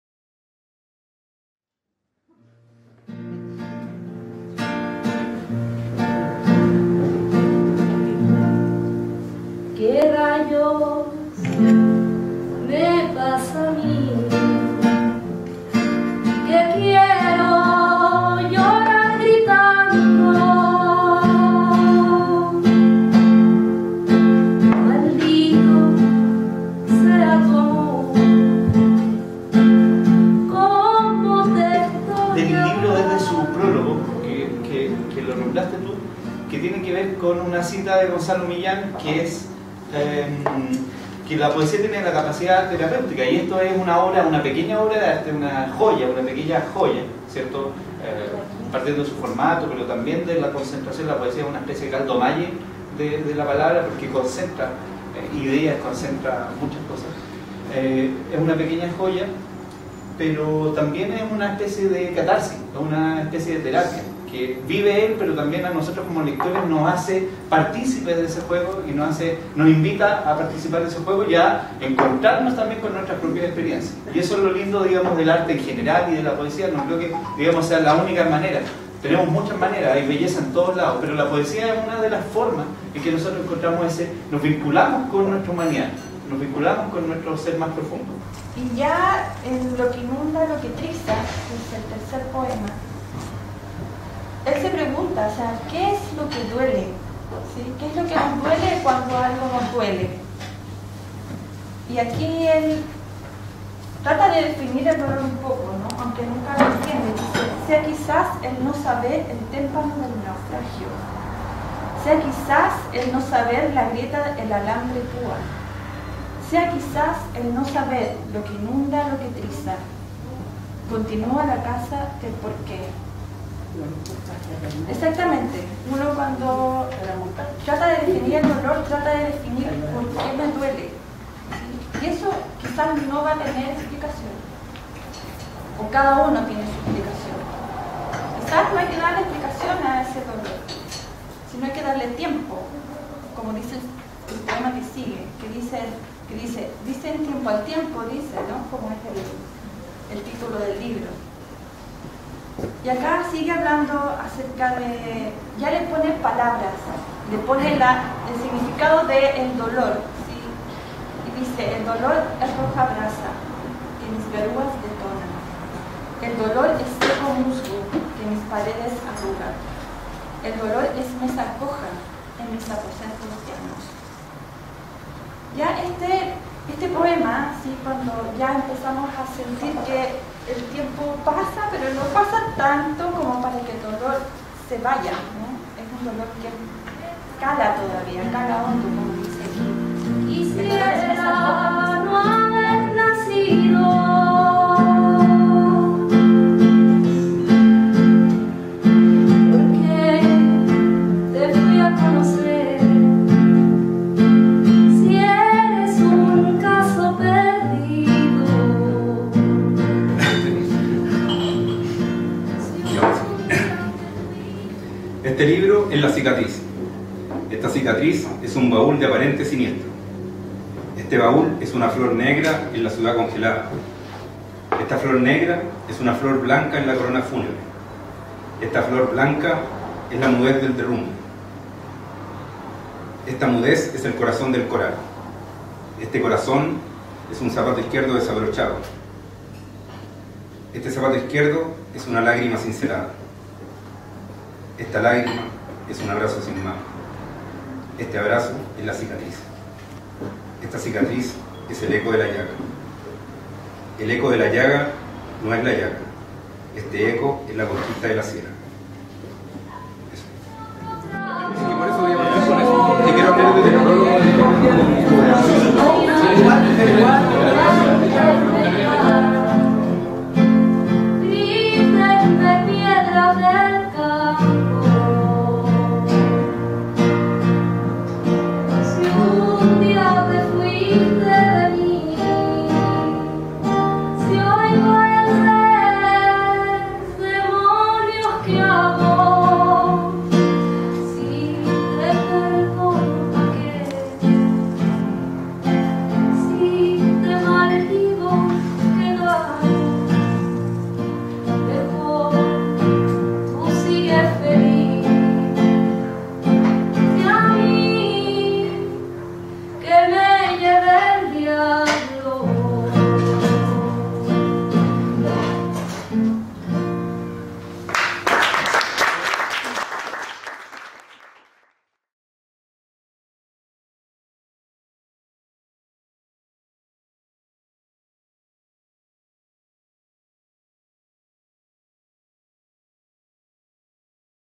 Lanzamiento libro "dicen tiempo al tiempo dicen"
Lanzamiento del libro de poesía de Leo Paredes: "dicen tiempo al tiempo dicen".
Realizado el 21 de noviembre en el Bar Turismo.